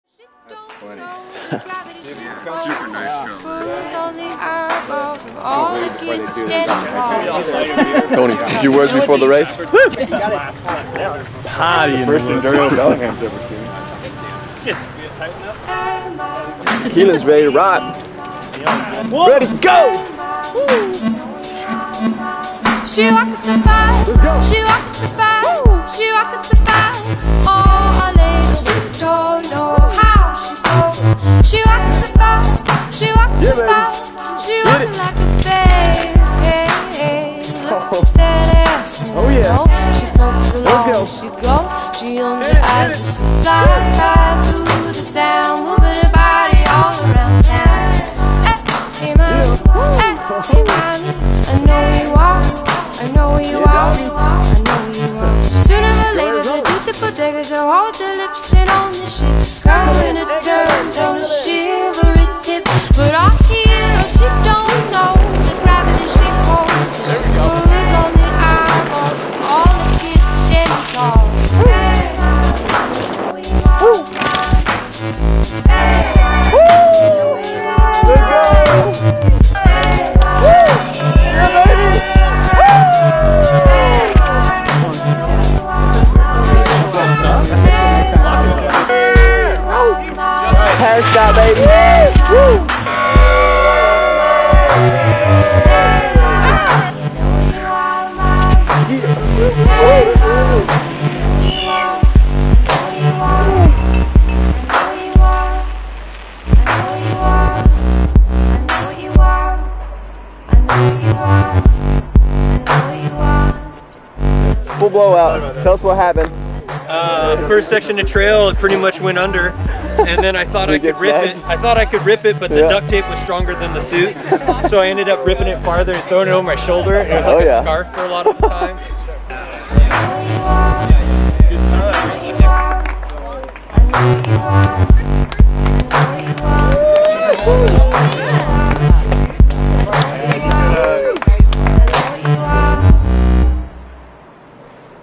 Klunker Worlds 2014
Klunker Worlds 2014 race edit

Once a year the OG's of the biker world come together and tear up the Lake Samish trails classic style. Loaded up with brewskis, twizzle sticks and their single speed-coaster brake-solid steel framed bikes these riders rip their way down for the chance to take home the Klunker world cup!

PARTY IN THE WOODS!